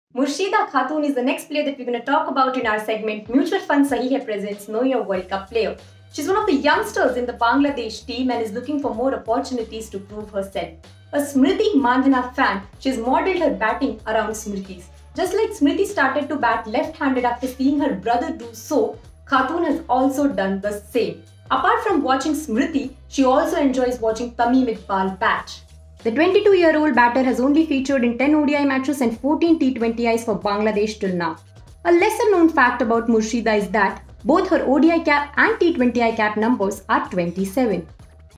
Know Your World Cup Player: Murshida Khatun
Murshida Khatun is the only left-hand batter for TeamBangladesh ever.

To know more about this 🇧🇩 cricketer ⤵️